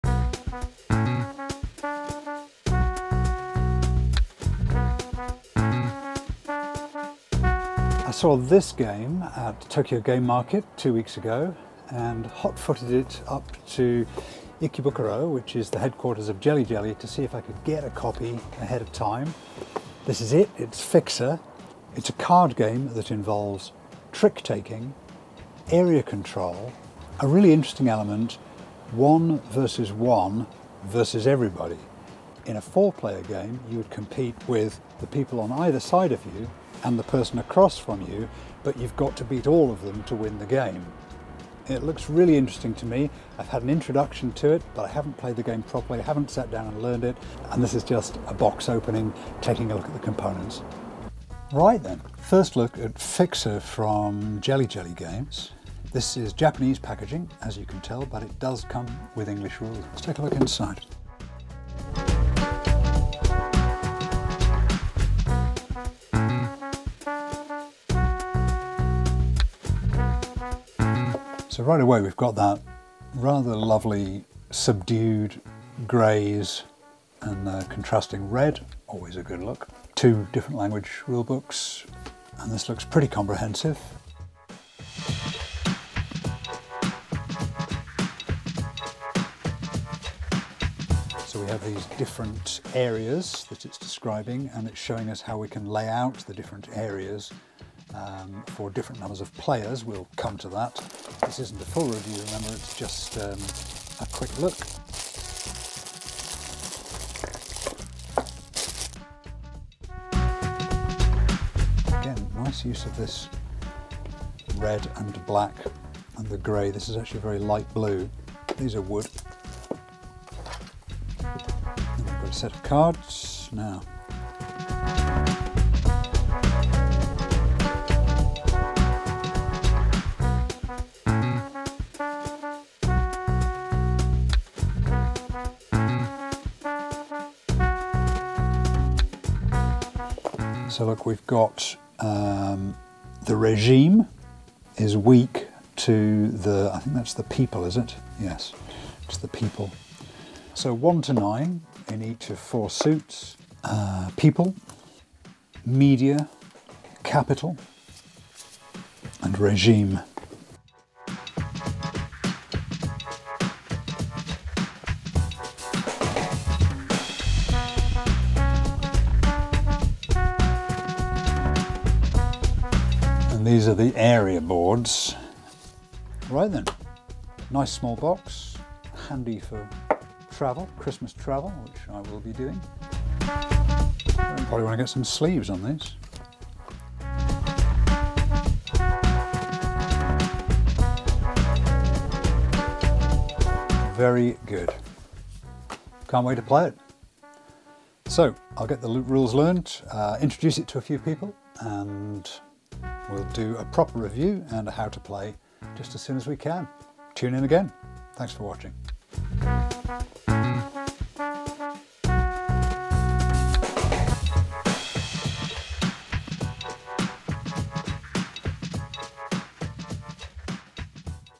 First Look at Fixer - an area control card game
A first look at Fixer by Jelly Jelly Games, their new whist-based and area-control game for 2-4 players, examining the packaging and components.

They say: "Let's manipulate capital, media, people and the regime to rule the country from the shadows. Judge areas of conflict carefully, sometimes deliberately losing an encounter to steal and exploit an opponent's card. This is a heavy card game that requires forward planning and cunning to outsmart the enemies"

So far we think it's very accessible, but can be very deep too. Check in again for a full review.